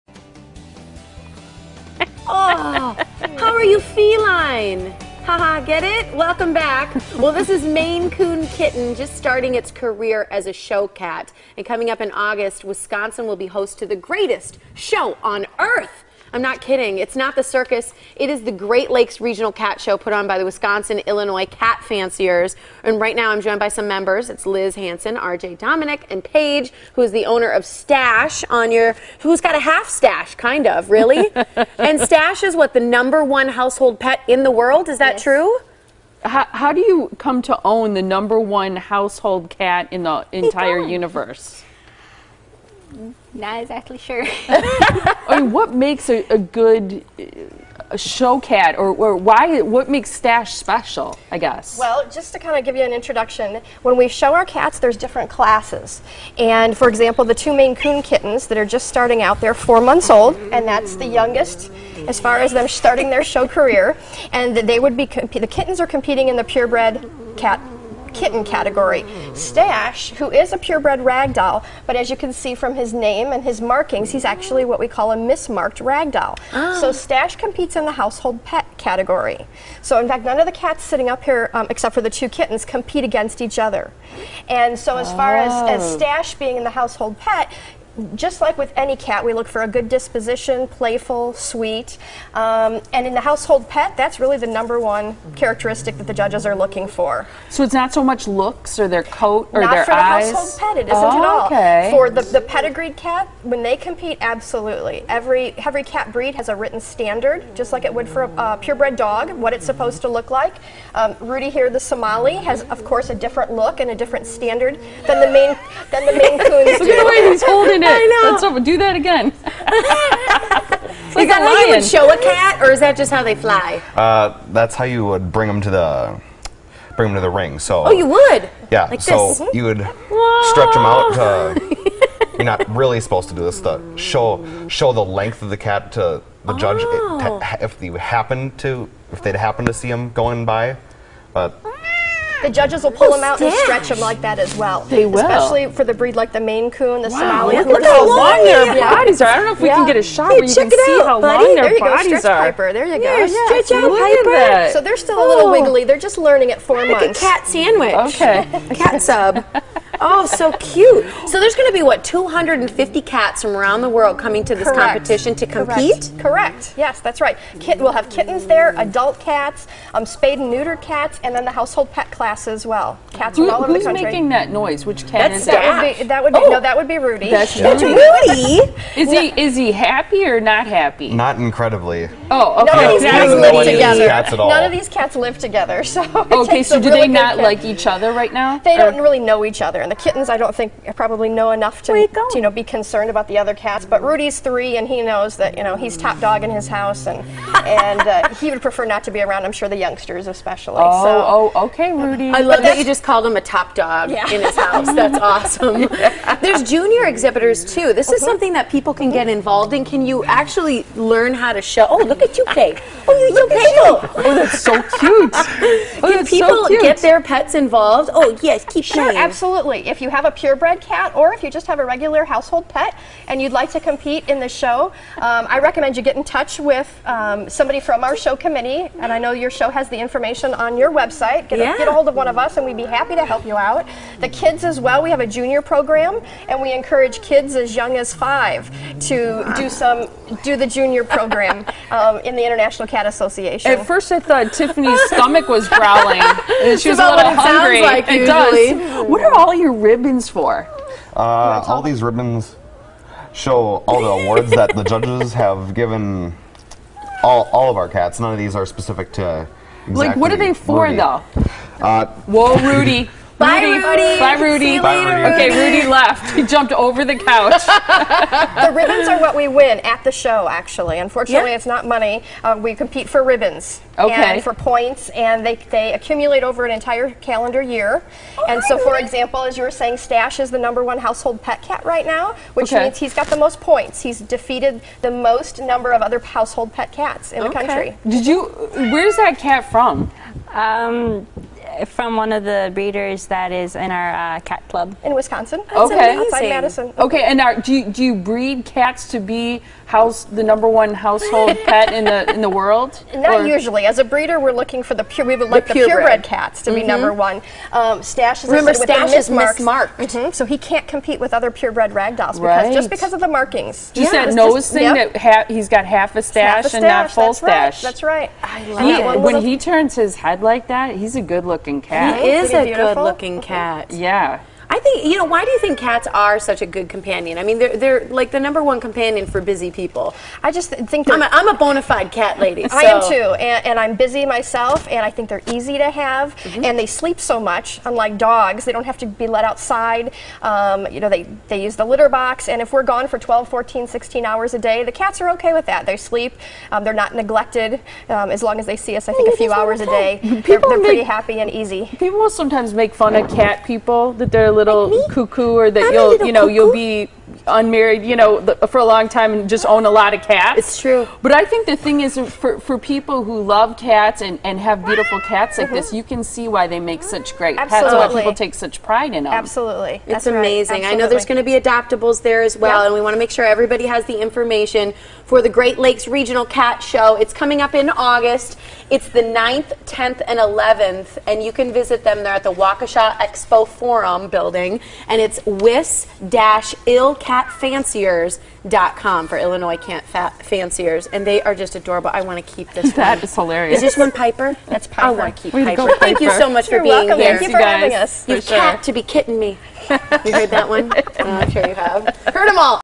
The Greatest Show On Earth
Liz Hansen, Exibitor and Breeder of Maine Coon cats, and RJ Dominick, Junior Exibitor Somali, Ragdoll and Maine Coon cats, joins us to discuss "The Greatest Show on Earth" featuring the best of the best in the feline world! Some of the top show cats in the country will be at this very special cat show. Go and cheer of your favorite cats as they show off for the judges!

The Wisconsin Illinois Cat Fanciers are proud to be hosting the TICA Great Lakes Regional Pedigreed & Household Pet Cat Show "The Greate